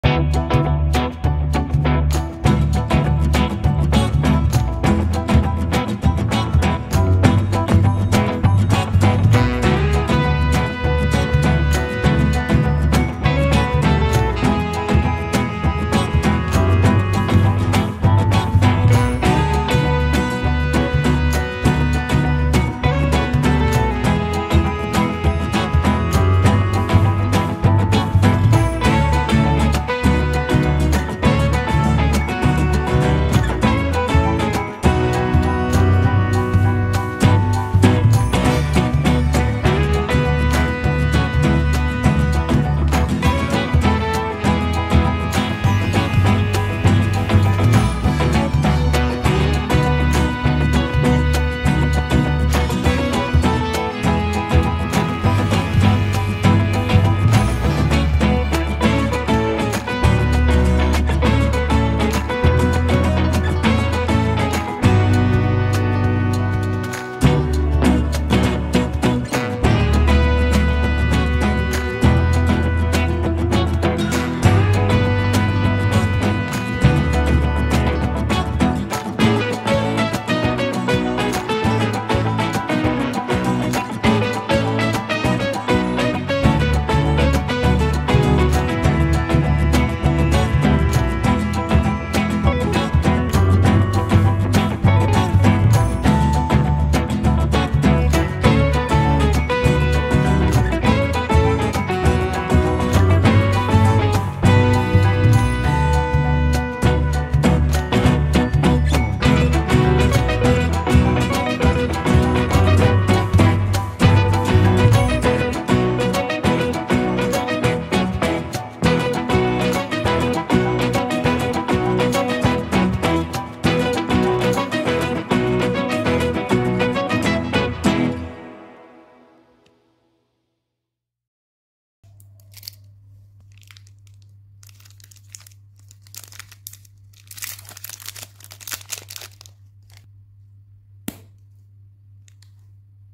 Lots of new candies opening ASMR, milka, kit kat and much more
unboxing lots of new candies, milka kit kat and much more candies .